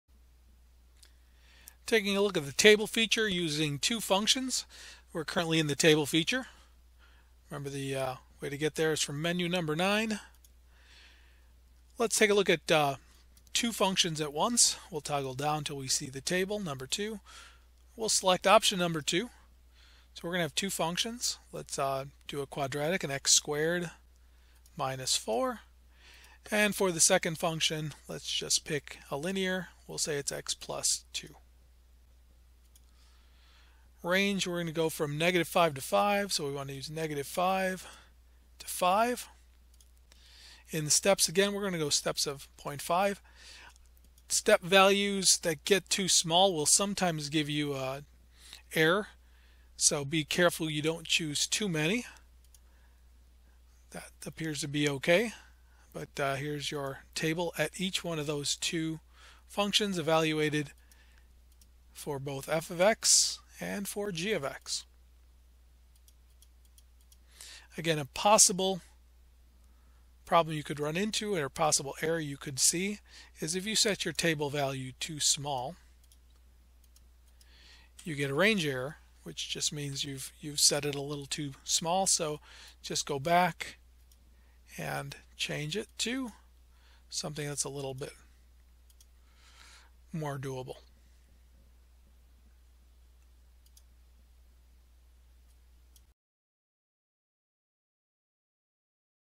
Tutorial Kalkulator ClassWiz - 5-7 Tabel Dua Fungsi
Video ini menunjukkan pengoperasian dasar CASIO ClassWiz.

[Model yang kompatibel ] fx-570EX, fx-991EX, fx-82EX, fx-350EX
[Topik Terkait] Tabel Dua Fungsi
[Kelas] SMA Kelas10 (Matematika Wajib)

ClassWiz adalah Kalkulator Ilmiah yang Tidak Dapat Diprogram Standar Kelas Baru dengan Teknologi LCD Resolusi Tinggi.